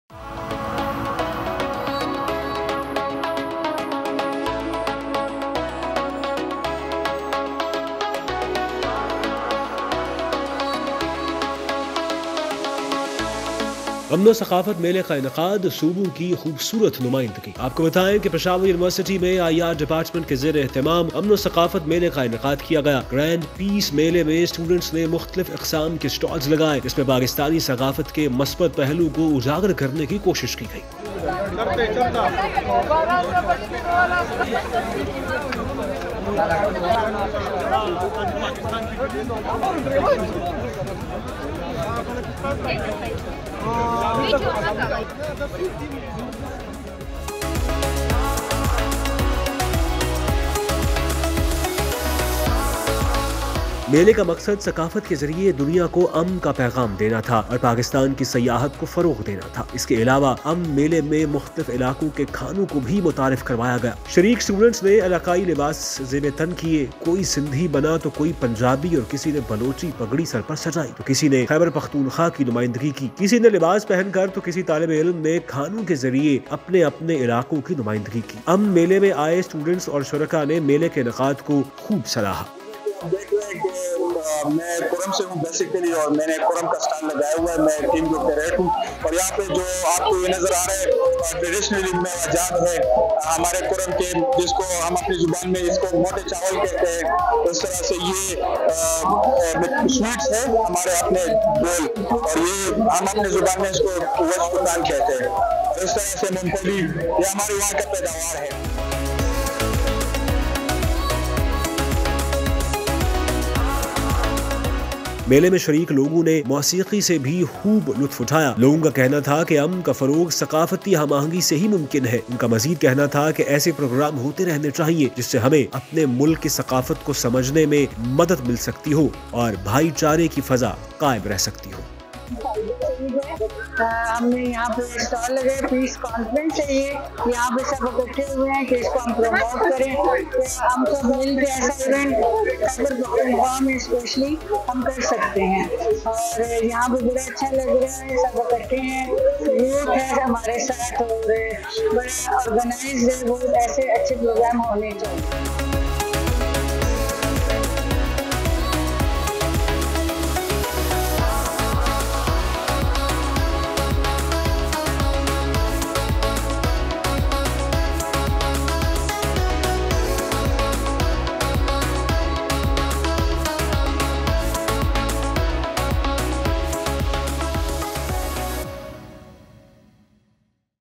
Organization of peace and culture fair. Beautiful representation of provinces | Pakistan Today
Organization of peace and culture fair. Beautiful representation of provinces

امن و ثقافت میلے کا انعقاد.صوبوں کی خوبصورت نمائندگی